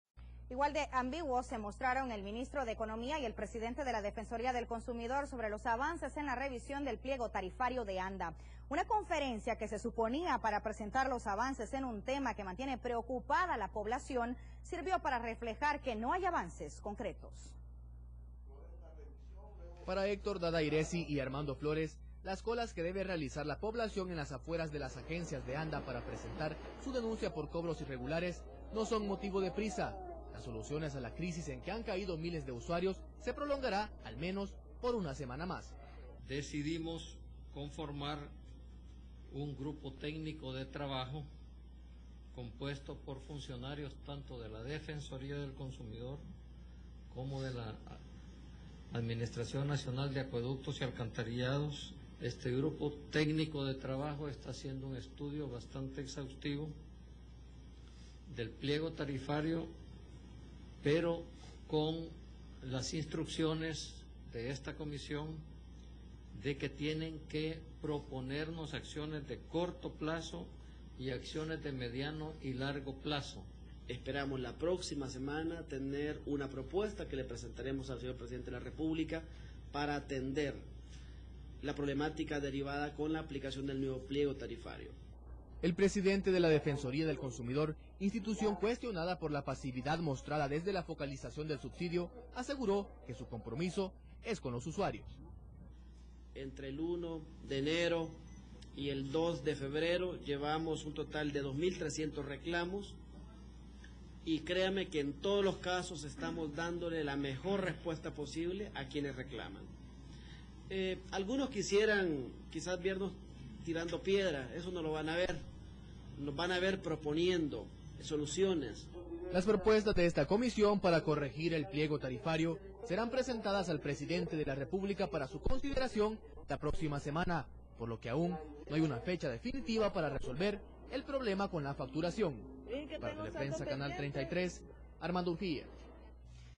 MINEC y DC opinan sobre pliego tarifario de ANDA 04-02-2010
Presentan informe de reunión de mesa interinstitucional que analiza pliego tarifario de ANDA
El Ministro de Economía,  Héctor Dada Hirezi junto al Presidente de ANDA, Marco Antonio Fortín y El Presidente de la Defensoría del Consumidor, Armando Flores, miembros de la Comisión de Gobierno que analizan los pliegos tarifarios de agua brindaron hoy detalles de las  primeras acciones que tomarán, como medidas de apoyo a los usuarios de ANDA.
El Ministro de Economía y Vocero oficial de la Comisión, informó que se ha creado un grupo técnico de trabajo conformado por miembros del Ministerio de Economía, ANDA, Defensoría del Consumidor y la Secretaría Técnica de la Presidencia.
Dentro de los avances en las reuniones informaron:
Se ha procedido a determinar acciones de corto plazo y acciones de mediano y